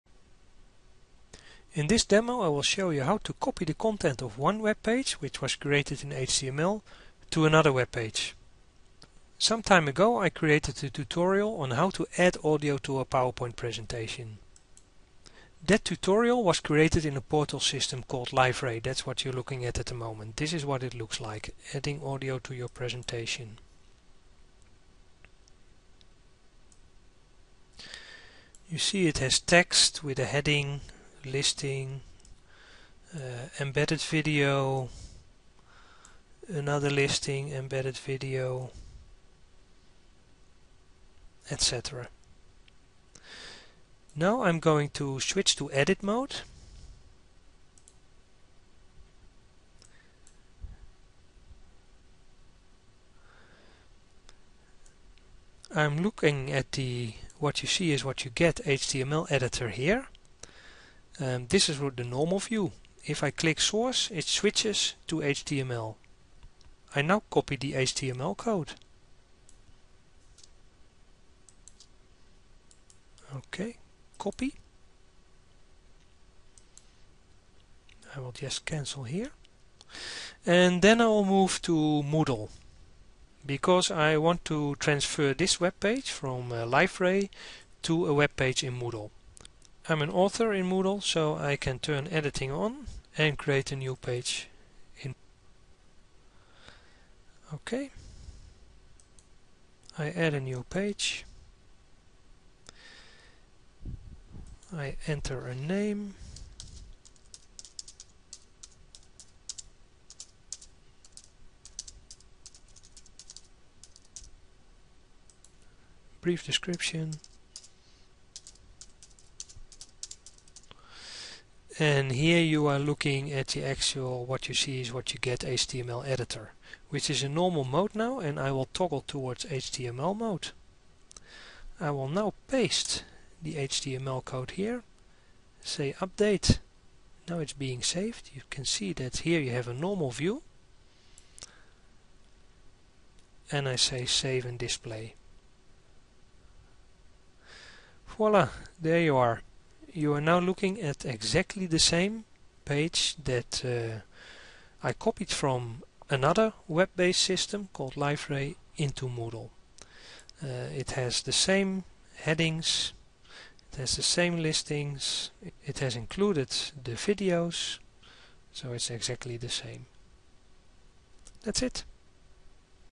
Copy and paste web page content in HTML
Copy the content of one page to another page between HTML editors